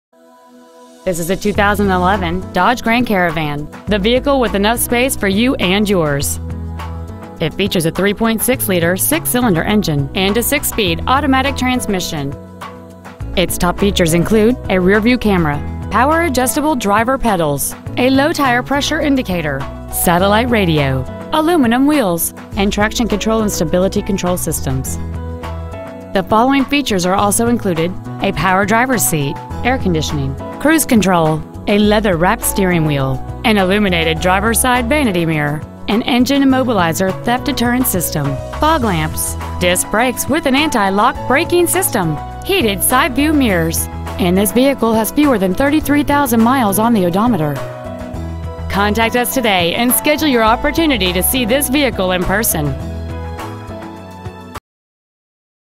2011 Dodge Grand Caravan Gainesville GA 30501
Year : 2011
 Make : Dodge
 Model : Grand Caravan
 Engine : Gas V6 3.6L/220
 Trans . : Automatic
 Exterior : Stone White
 Miles : 32,921
 Interior : BLACK
 HARDY CHEVROLET INC
 2115 Browns Bridge Road
 Gainesville , GA 30501
 2011 Dodge Grand Caravan Gainesville GA